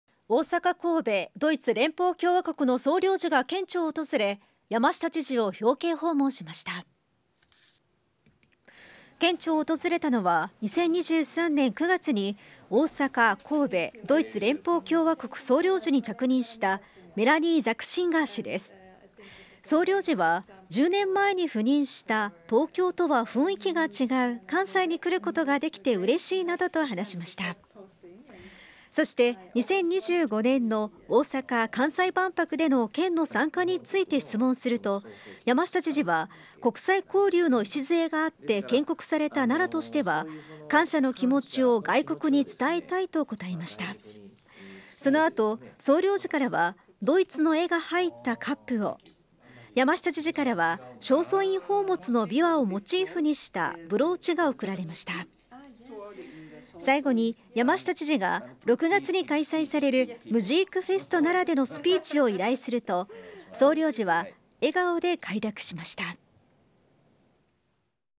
20240204-01 大阪・神戸ドイツ連邦共和国総領事が山下知事を表敬訪問
大阪・神戸ドイツ連邦共和国の総領事が県庁を訪れ山下知事を表敬訪問しました。